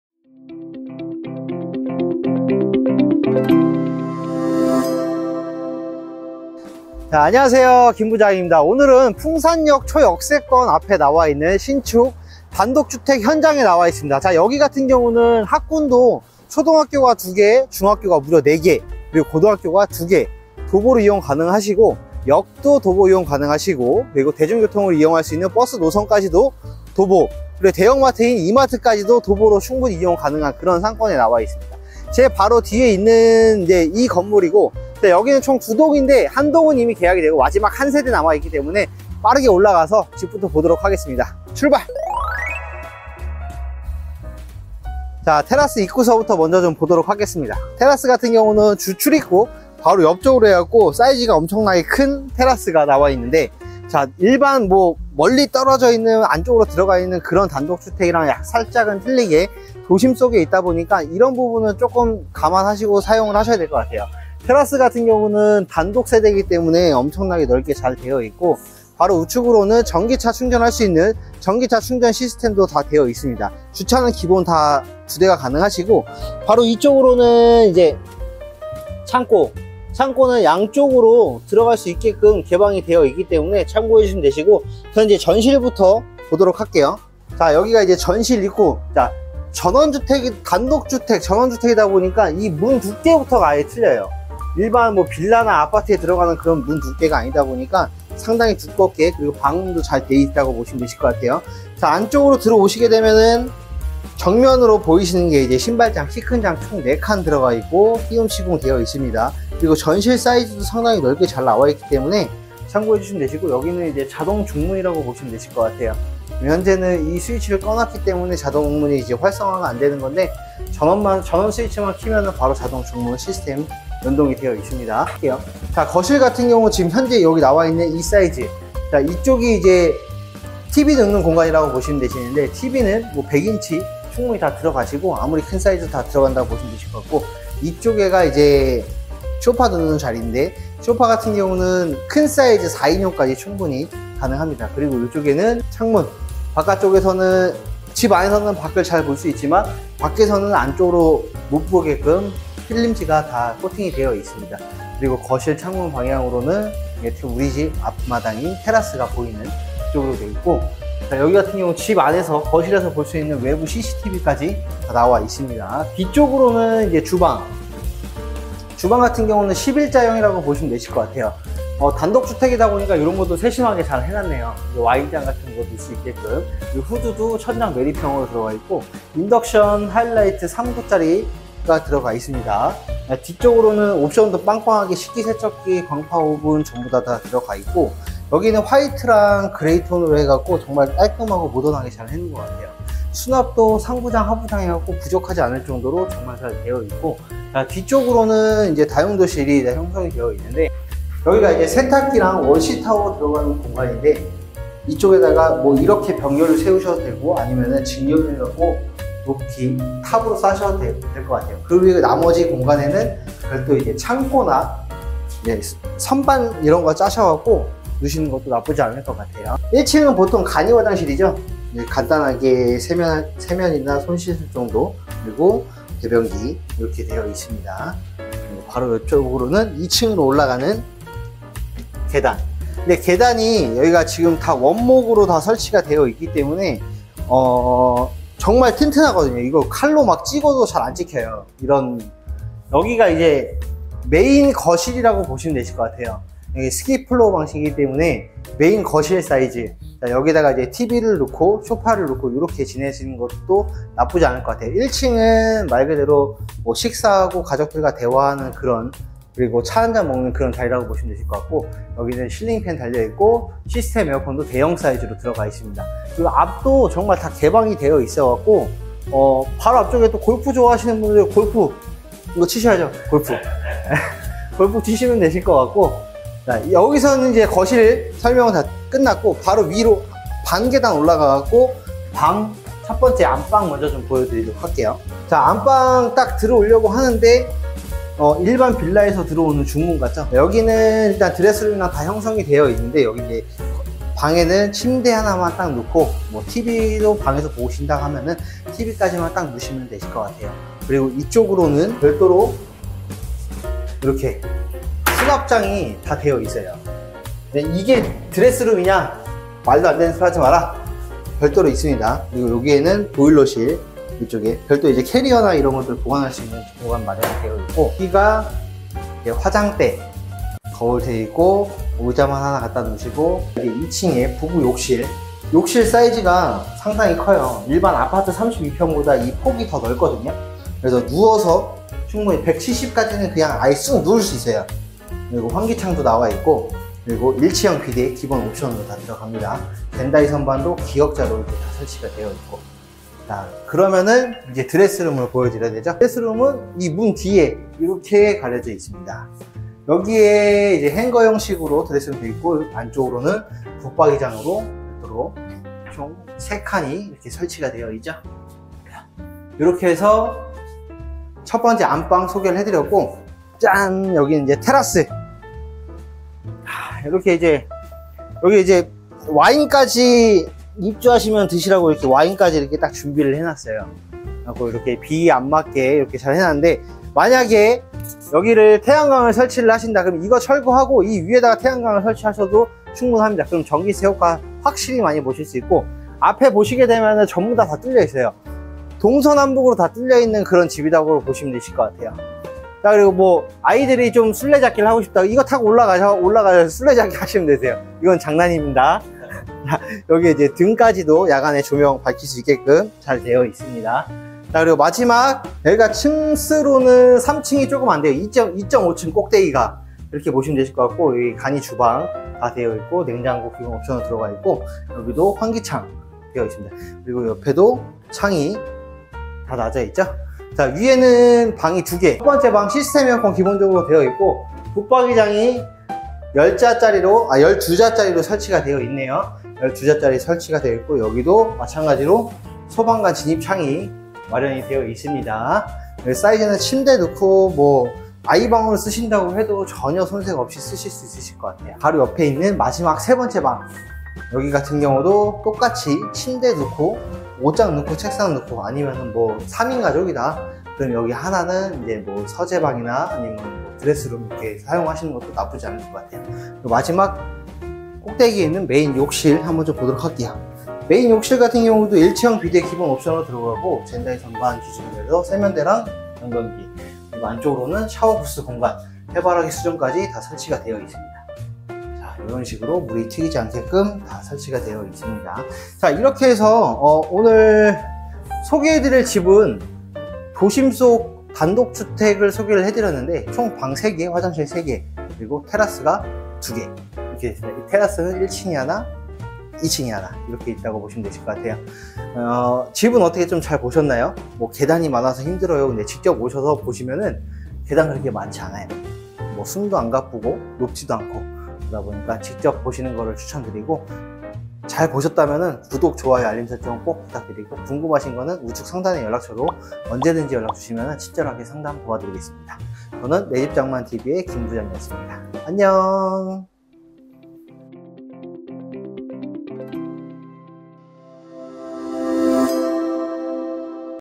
[일산단독주택] 풍산역 초역세권에 위치한 잔여 1세대! 1층에는 전용주차장과 마당이~ 3층에는 프라이빗테라스가 있는집! 아이키우기 좋은 위치! [고양시단독주택][일산전원주택]
자세한 내용은 더보기 클릭 ☞

[일산단독주택]
풍산역 초역세권에 위치한 잔여 1세대!
1층에는 전용주차장과 마당이~
3층에는 프라이빗테라스가 있는집!
아이키우기 좋은 위치!
[고양시단독주택][일산전원주택]

▷ 위        치 : 경기도 고양시 일산동구 풍동
▷ 종        류 : 단독주택
▷ 건축구조 : 철근콘크리트
▷ 분양세대 : 총 2세대 (1세대 분양완료)
▷ 준        공 : 2022년 8월 준공완료
▷ 분  양  가 : 12억대
▷대지면적 : 148㎡ (44.77평)
▷실내면적 : 143.36㎡ (43.37평)
▷ 실입주금 : 문의
▷ 구        조 : 1층 거실, 주방, 화1, 전용주차장, 테라스 (13평)
                        2층 메인거실 (20평)
                        3층 안방, 외부테라스 (12평)
                        4층 다락, 보조주방, 방2
▷ 옵        션 : 시스템에어컨 5대 (거실2, 각방, 다락)
                        실링팬
                        하이라이트
                        아일랜드식탁
                        욕조
                        식기세척기
                        냉장고
                        광파오븐
                        드레스룸, 붙박이장식장
                        공기순환기
                        CCTV
                        자동센서중문
▷ 특이사항 : 잔여 딱 1세대 남은 현장!
                        풍산역, 이마트 도보로 이용 가능!
                        학군밀집지역 아이키우기 매우 좋은 위치
                        초등학교 2 / 중학교 4 / 고등학교 2
                        4층짜리 아주 근사한 집입니다~

📡내집장만TV는 직접 시행, 시공, 분양을 하는 업체로써 고객님이 원하는 니즈를 정확하게 파악하여 내집마련을 하시는데에 보탬이 되겠습니다.

[일산단독주택][일산타운하우스][일산전원주택]